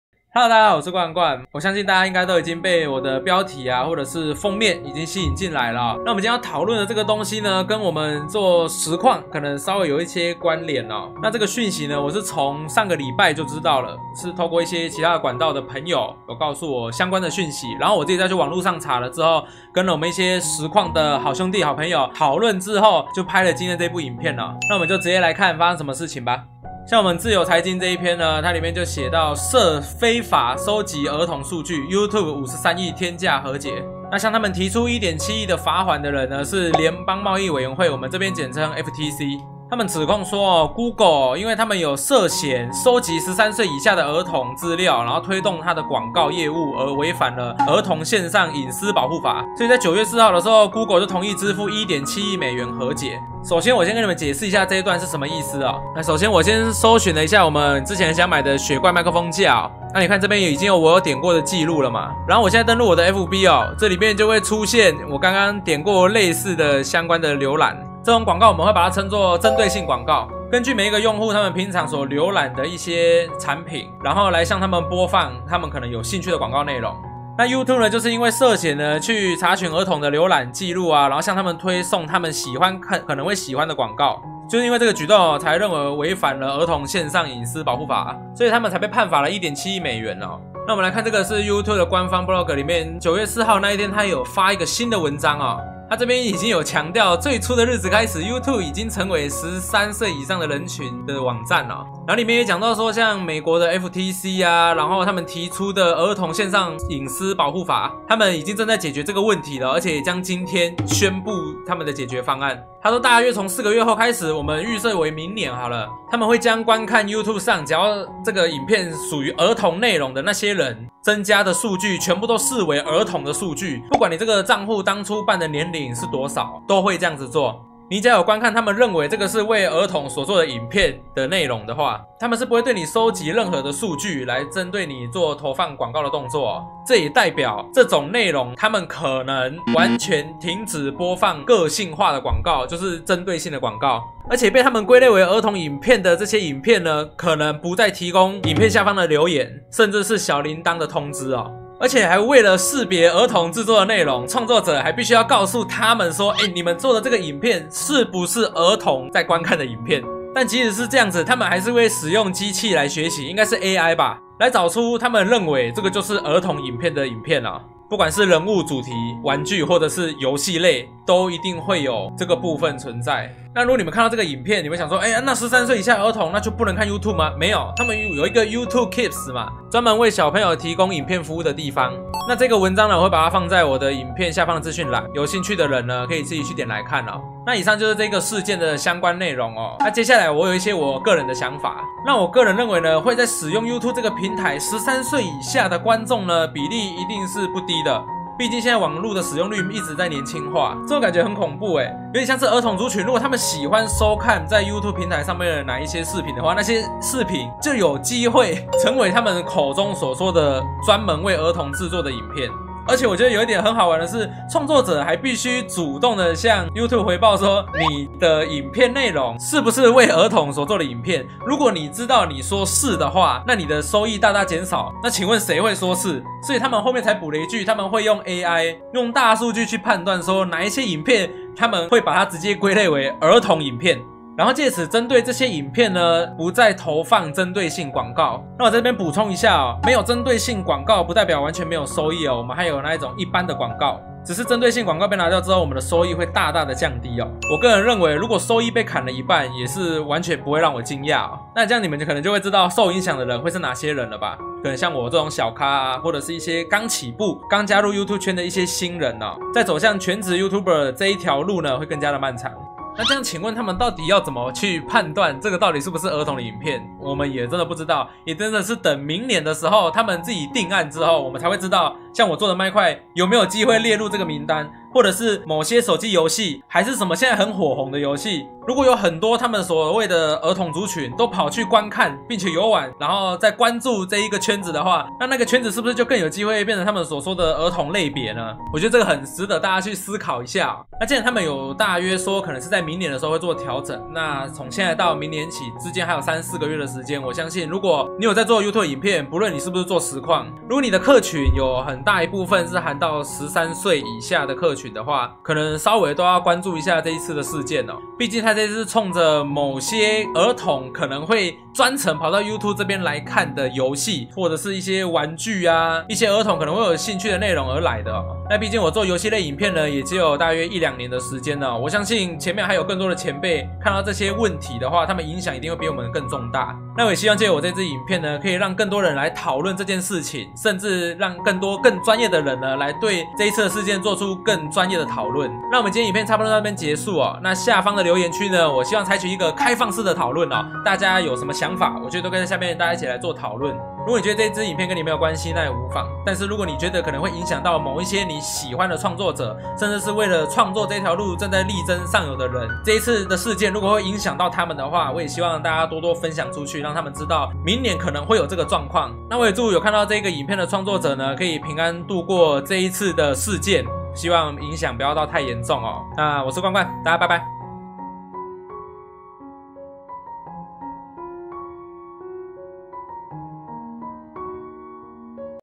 【冠冠】明年YouTube實況圈創作者會減少?YT這個決策....我會不會失業?【廣告收益大縮減】
錄影軟體:OBS
攝影鏡頭:羅技C922

主角:冠冠本尊